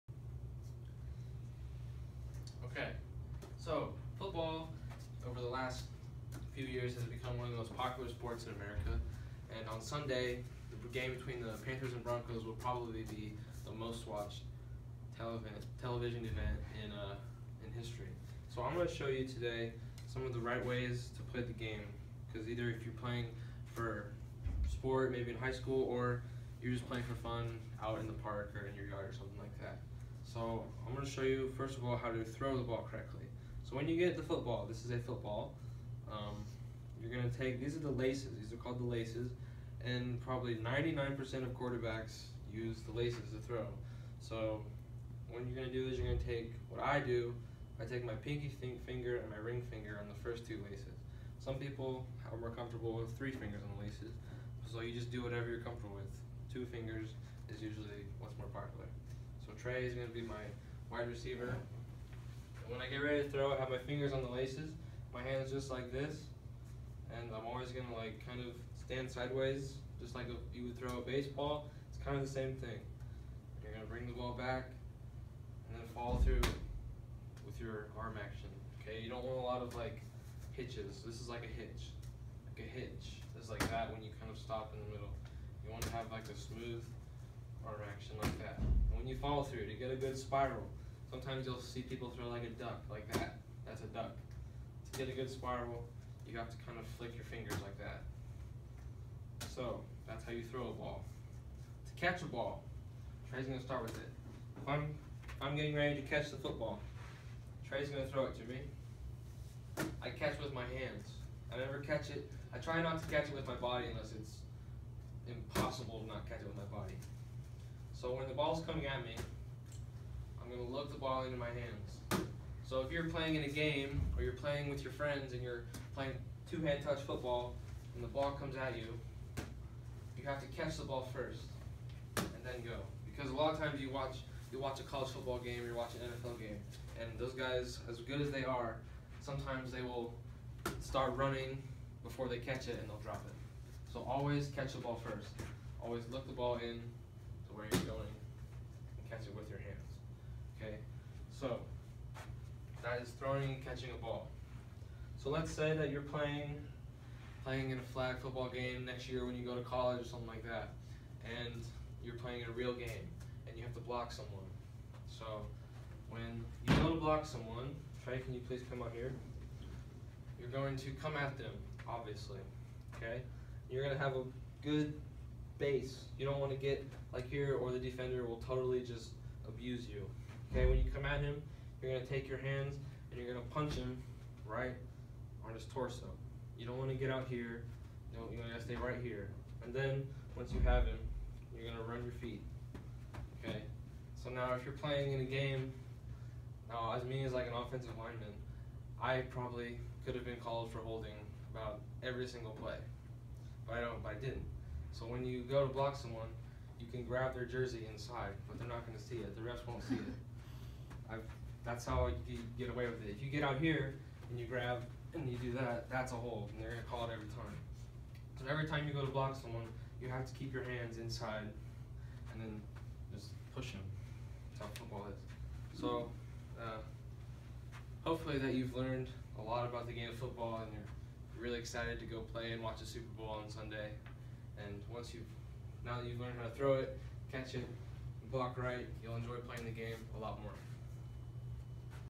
Demonstration Speech
Caden Byers giving his Demonstration speech for Hutchinson Community College.